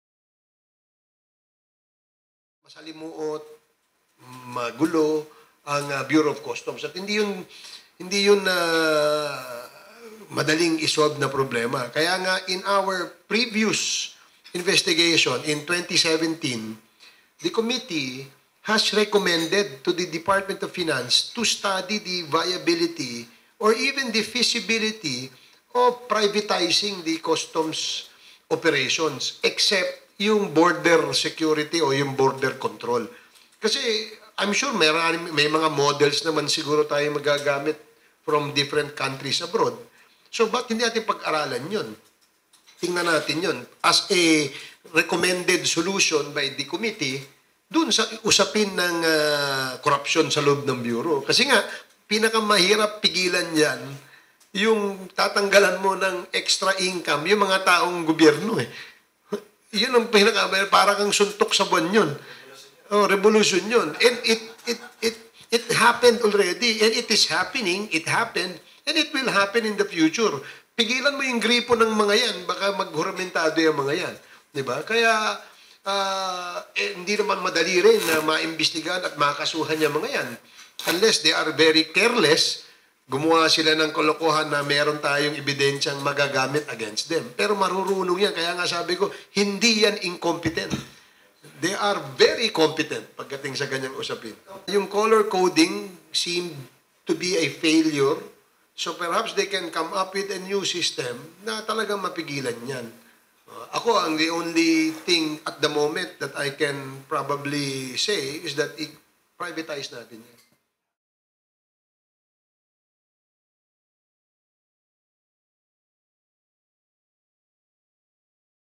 Barbers eyes Customs' privatization to Dept. of Finance
Video and editing by RYAN LEAGOGO/INQUIRER.net